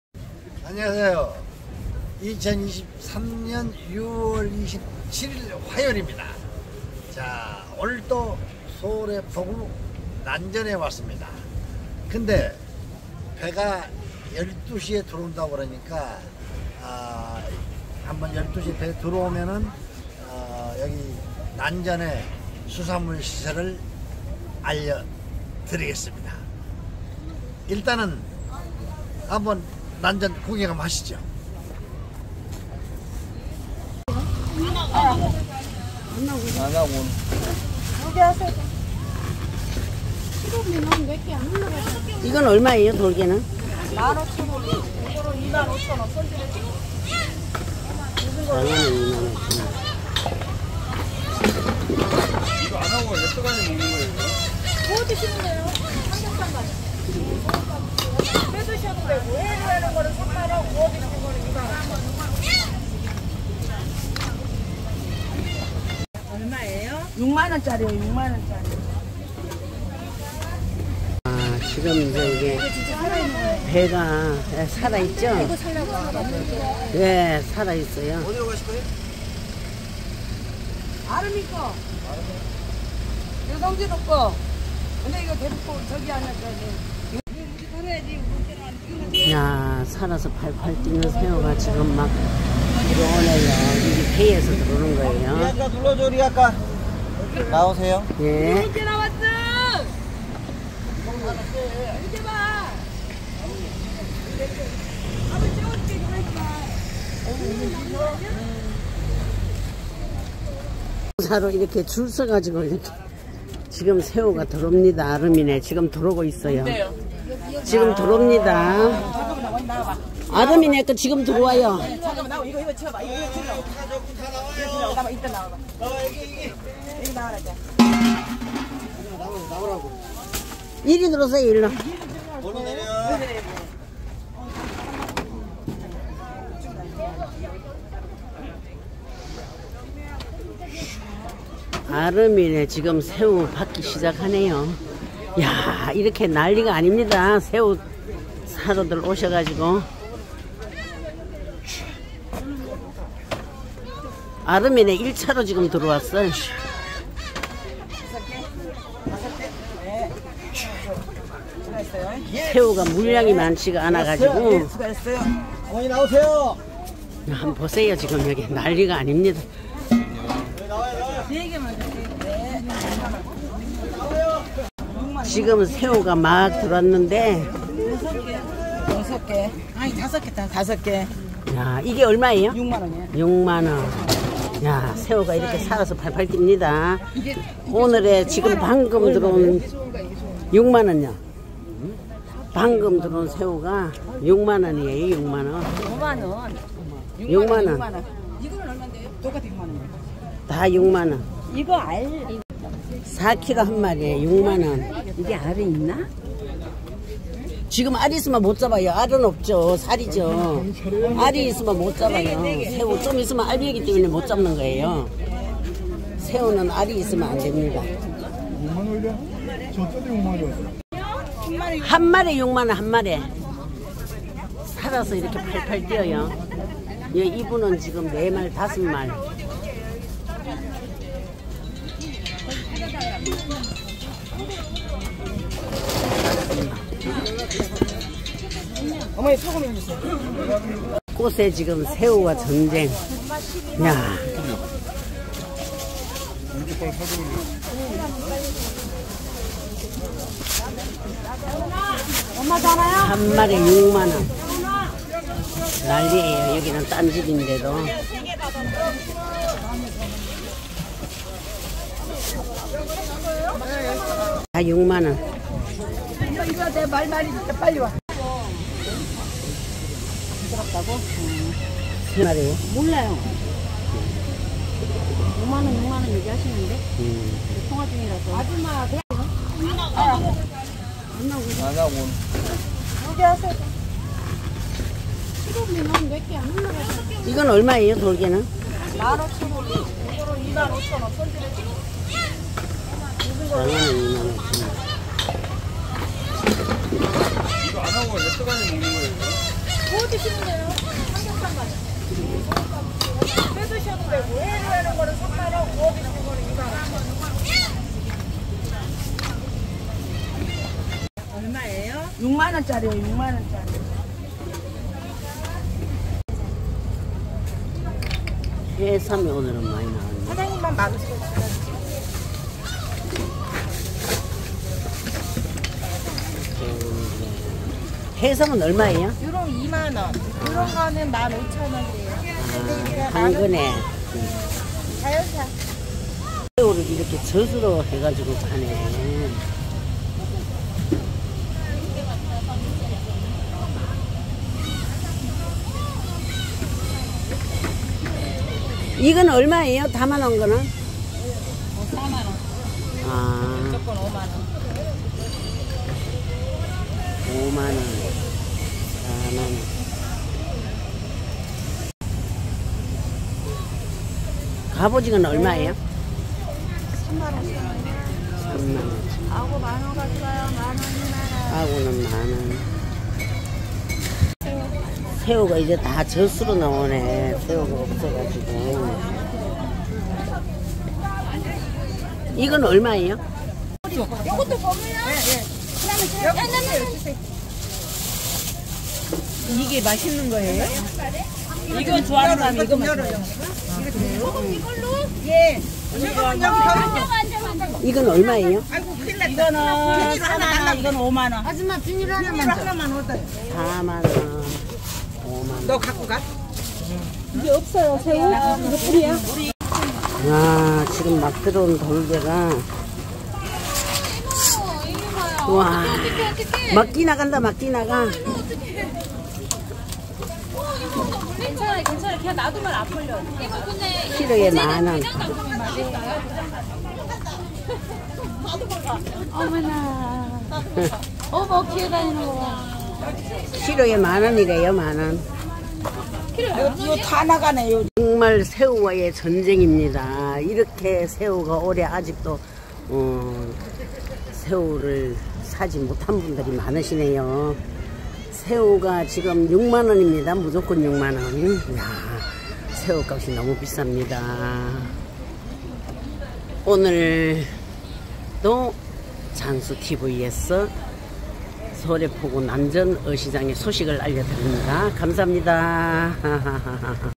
새우가엄청비싸면서도 물량이딸려서 줄을서있어도 못살정도네요!
소래포구난전어시장#새우돌게새우젓#해삼#